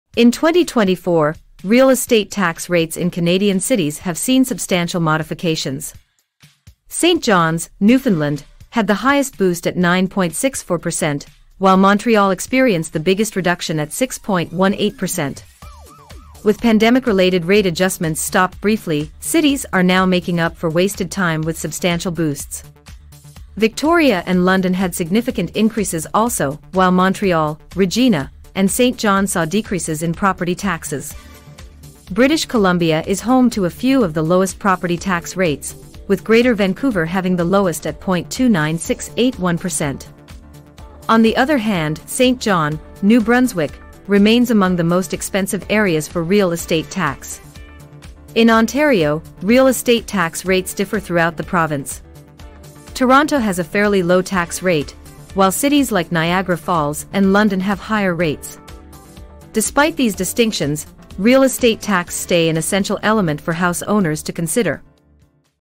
Discover the ranking of Canada's most & least affordable property taxes in 2024
In 2024, real estate tax rates in Canadian cities have seen substantial modifications. St. John's, Newfoundland, had the highest boost at 9.64%, while Montreal experienced the biggest reduction at 6.18%. With pandemic-related rate adjustments stopped briefly, cities are now making up for wasted time with substantial boosts.
British Columbia is home to a few of the lowest property tax rates, with Greater Vancouver having the lowest at.29681%.